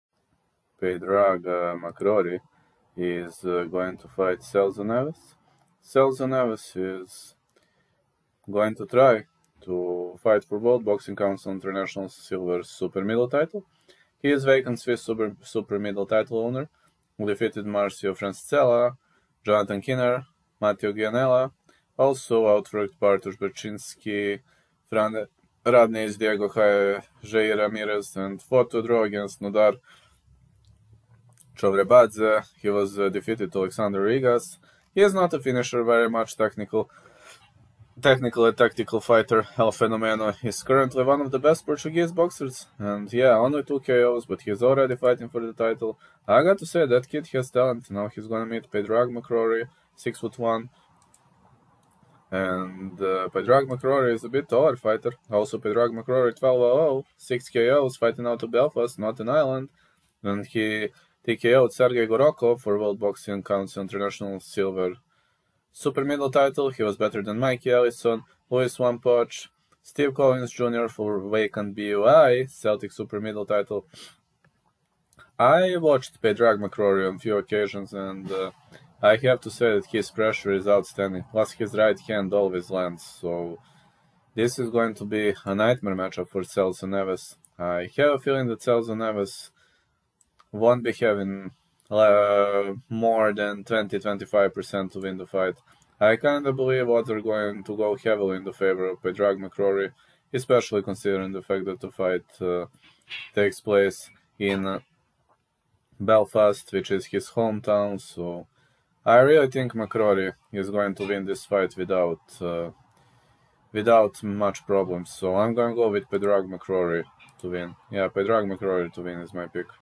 Padraig McCrory Vs Celso Neves - Prediction
The Bulgarian Cowboy picks Padraig here.